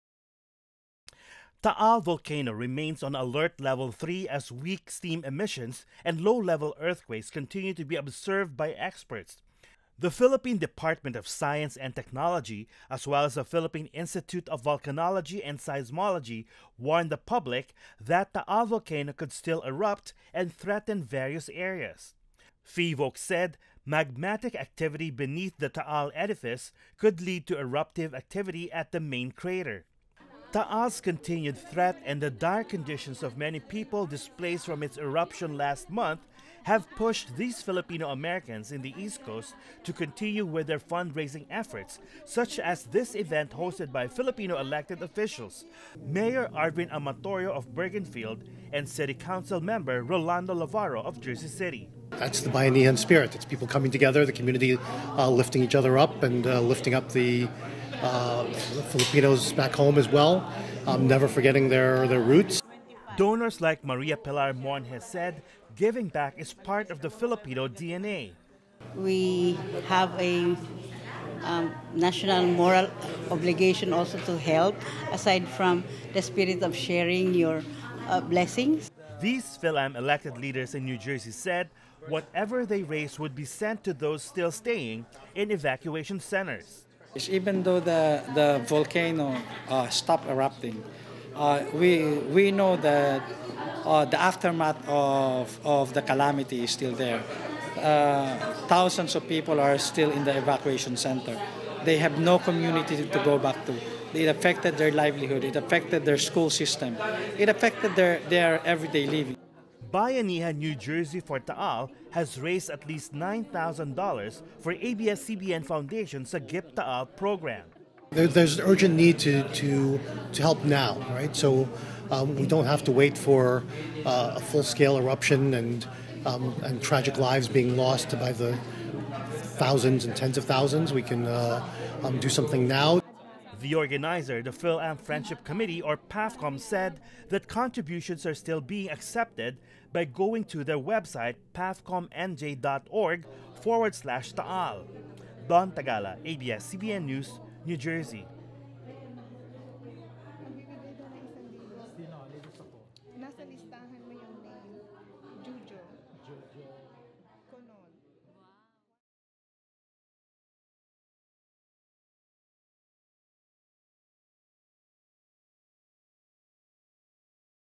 New Jersey Filipinos raise money for victims of Taal volcano eruption
TaalVolcano’s continued threat and the dire conditions of many people displaced from its eruption last month have pushed these Filipino-Americans in the East Coast to continue with their fundraising efforts, such as this event hosted by Filipino elected officials, Mayor Arvin Amatorio of Bergenfield and city councilmember Rolando Lavarro of Jersey City.